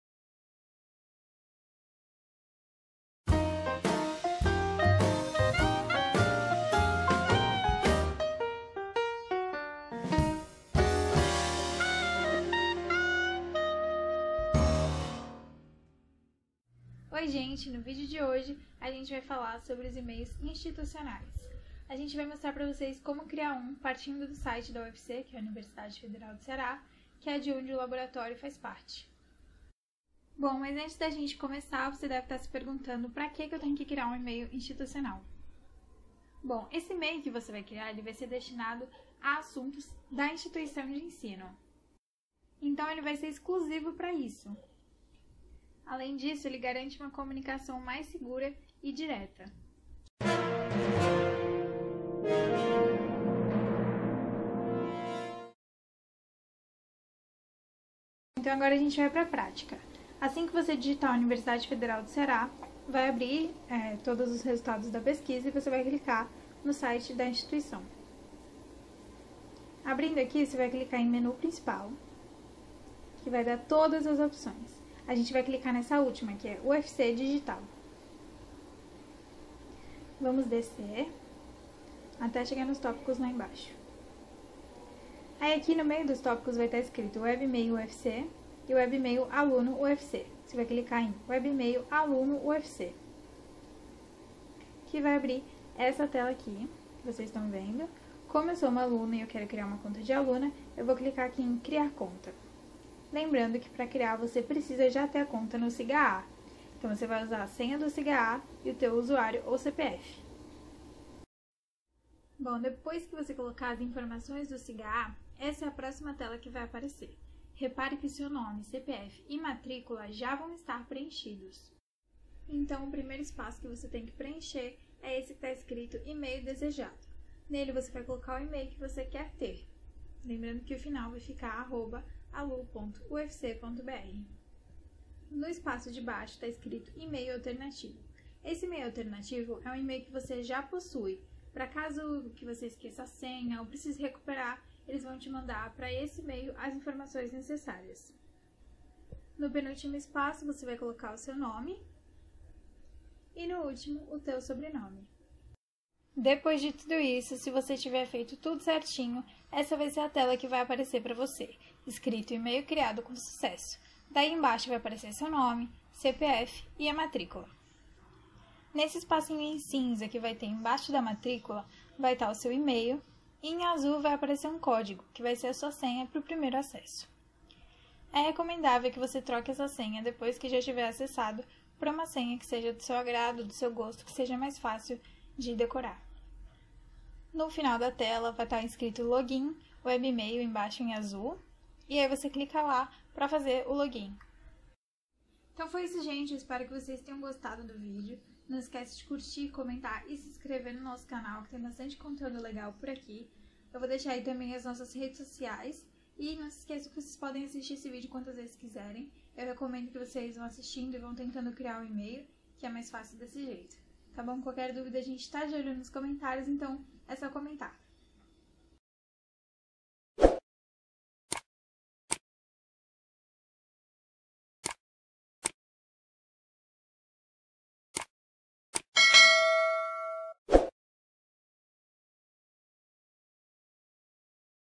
Email Institucional UFC, como criar?
Passo a passo de criação do Email Institucional da Universidade Federal do Ceará.
Dúvidas podem ser encaminhadas através dos comentários.